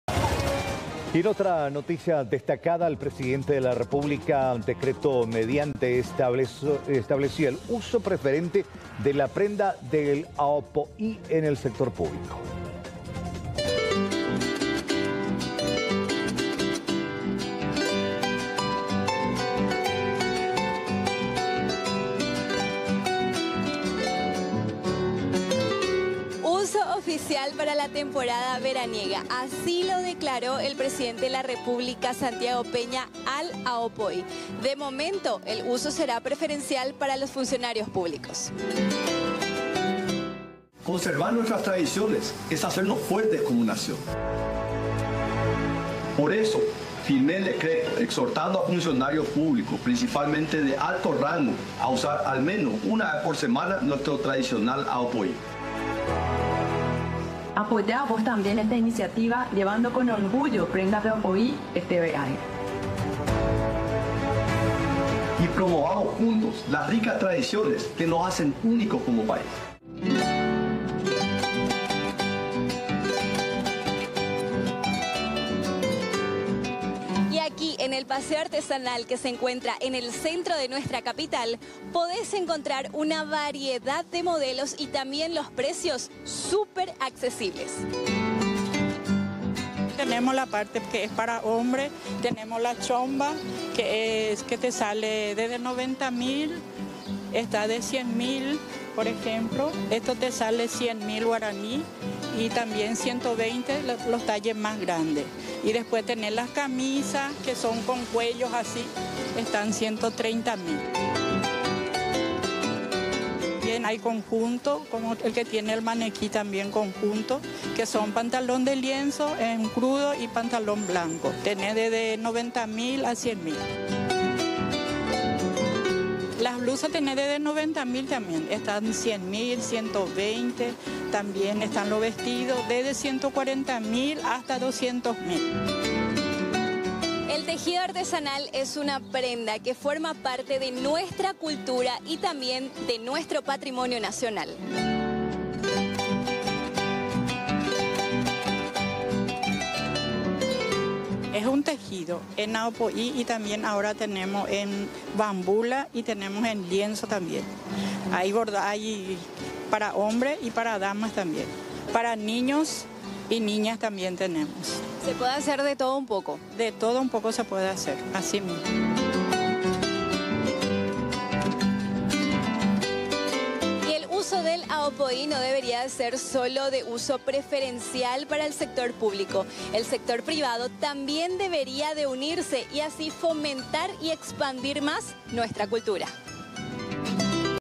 Ao po´i la prenda tradicional para este verano
El tejido de ao po´i es una prenda artesanal característica de la cultura paraguaya, el ejecutivo declaró como prenda oficial para la temporada de verano mediantte el decreto 929.